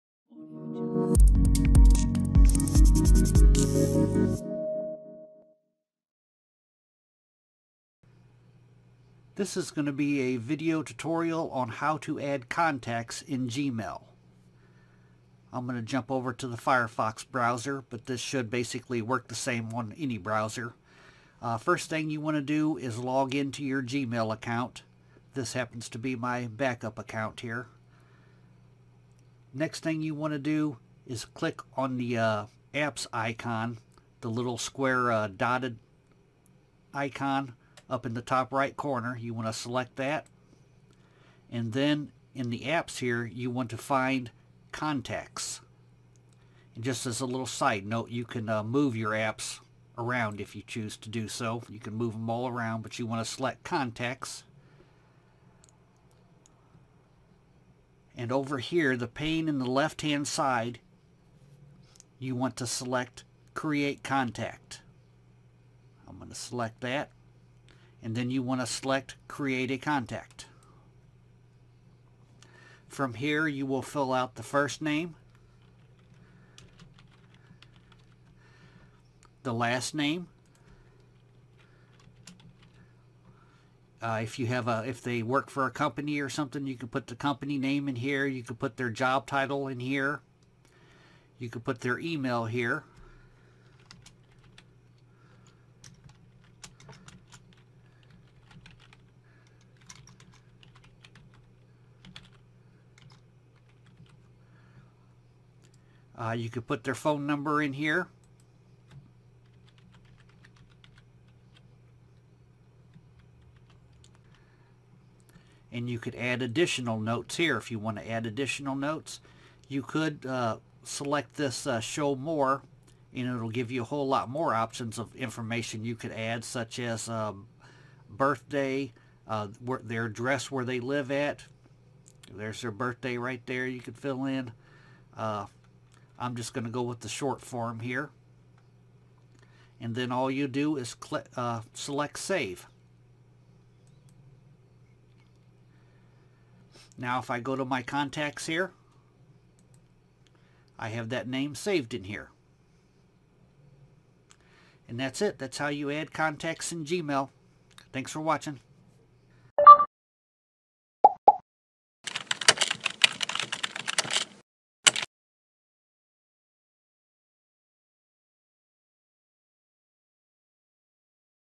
Google Gmail: How To Add New Contacts From A Desktop or Laptop
This is a video tutorial on how to create folders in Google Gmail using a desktop or laptop

In this tutorial, I will show how to add contacts to your Gmail account.
1. sign on to your Gmail account.
2. In the upper right corner, click the icon with dots (Google Apps)
3. In the top right corner, click "Create Contact"
4. Click Create Contact again.
6. Save!

Chapters
0:00  How To Add Contacts In Gmail
0:20  Sign into Gmail
0:31  Click on the Apps Icon and locate "Contacts"
0:57  Choose "Create Contact"  from the left pane.
1:12  Fill Out Contact Information
2:20  Select "Save"